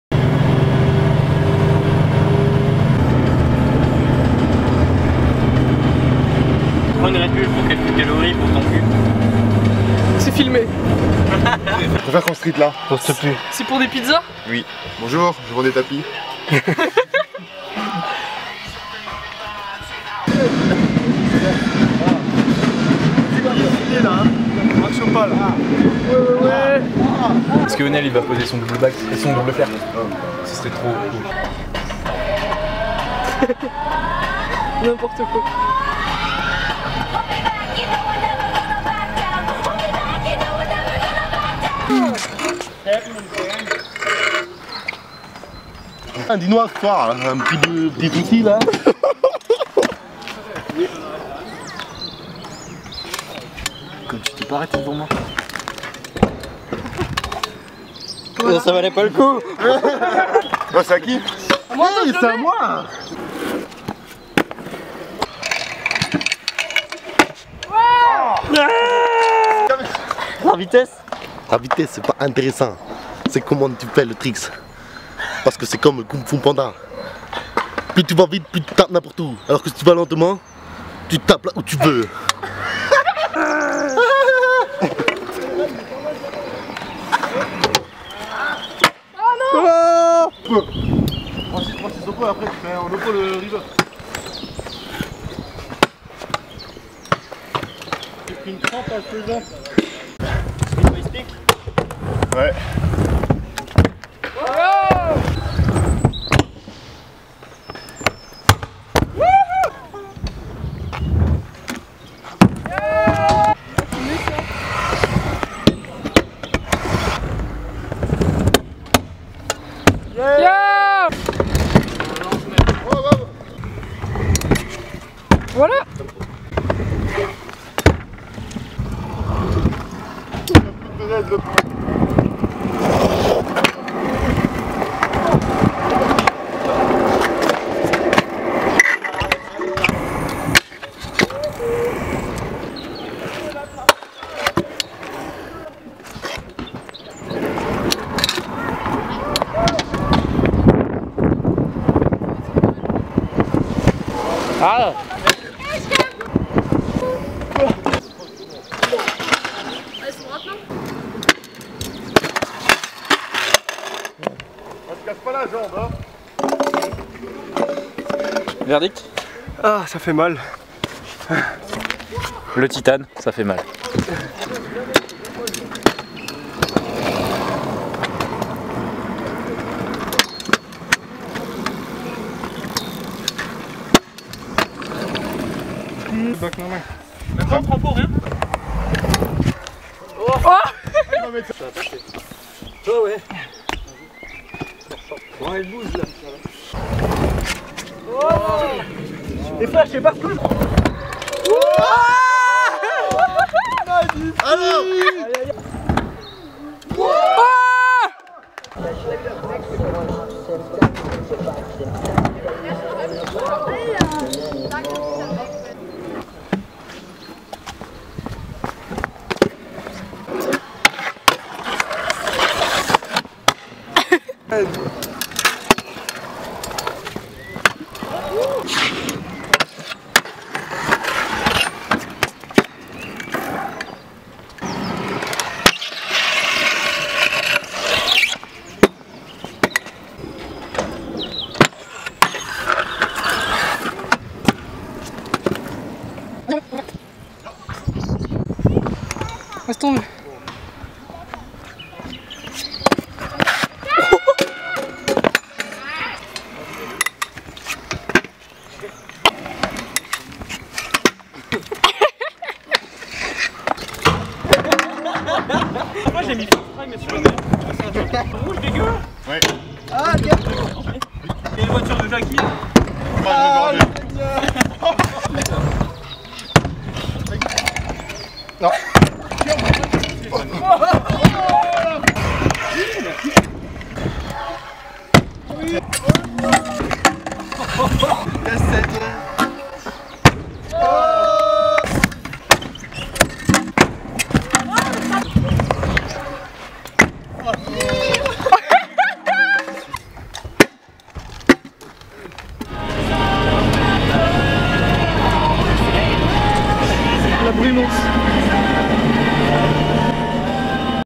Webisode #7 | Waldkirch, Germany
Une grosse session chill entre potes en Allemagne, comme on n'en montre plus assez. Partagez!

Featuring : Médi Bechiri, Bouli, Victor Formaniak, Jésus, Antoine Lucot, Valentin Schultz, Stéphane Vogt, Pierre Wegmann, Juls Yapa, LB Zemb et Inès Jourdain